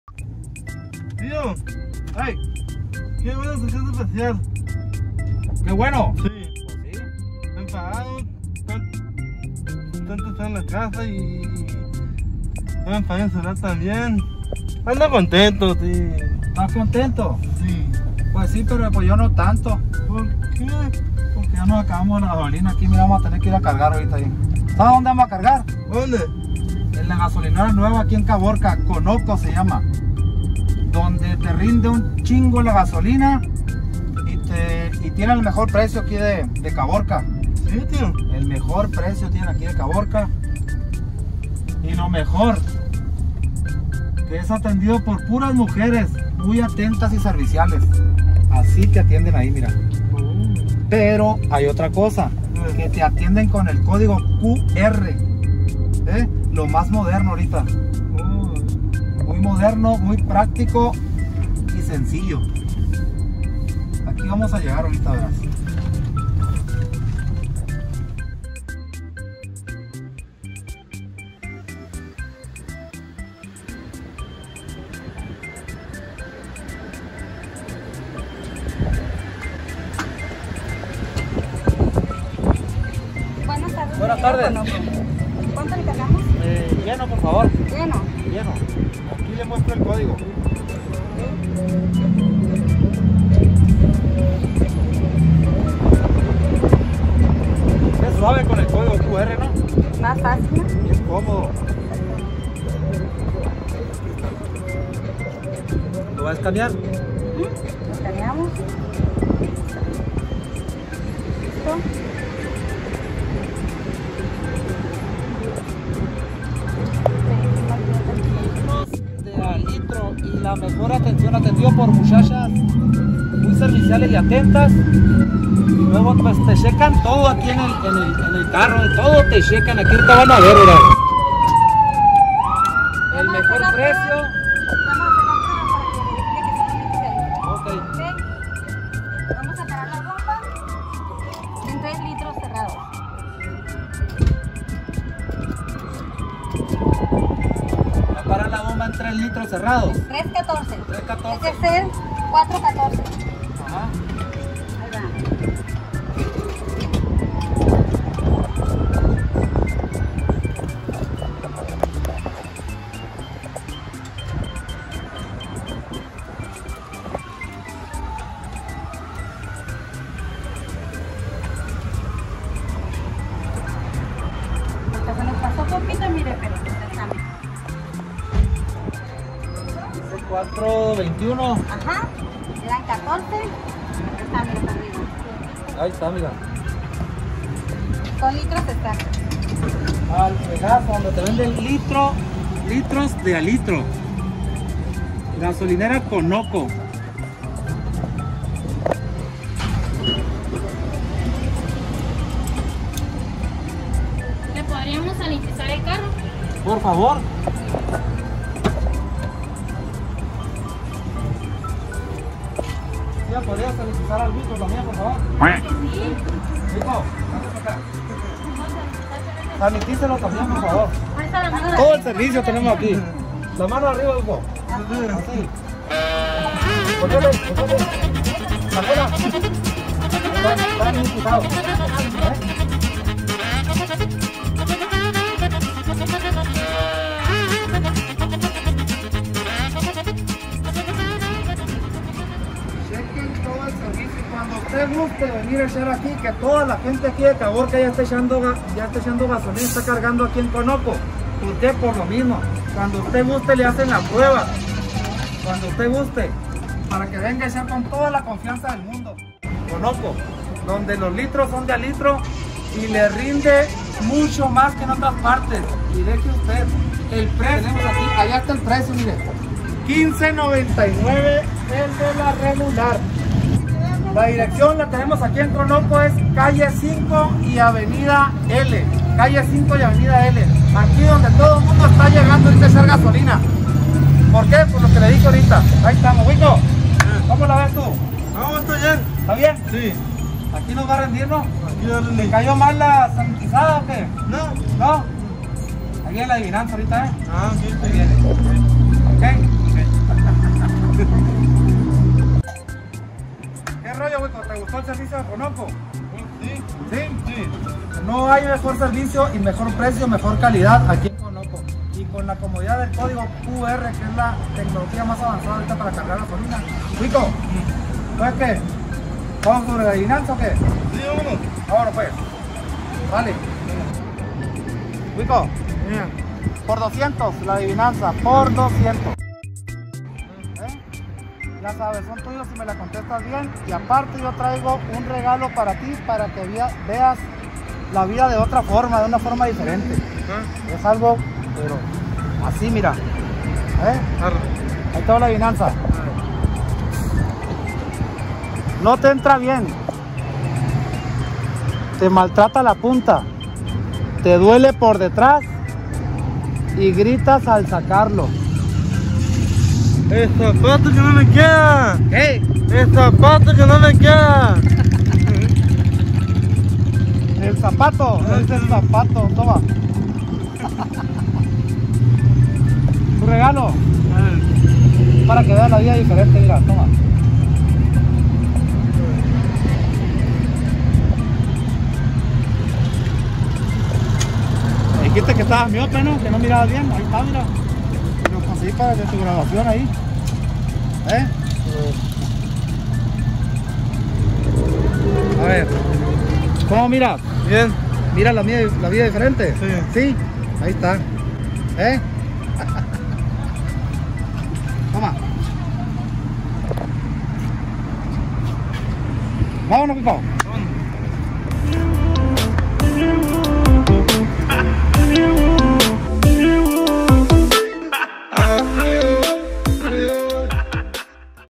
EL KUATE Y EL WIKO con adivinanzas EN GASOLINERA CONOCO !
Raza nos toco hacer unas adivinanzas en LA NUEVA GASOLINERA DE CABORCA, en CONOCO , SI TU ERES DE CABORCA SONORA no dejes de Comprobar que aqui SI SON LITROS DE A LITRO.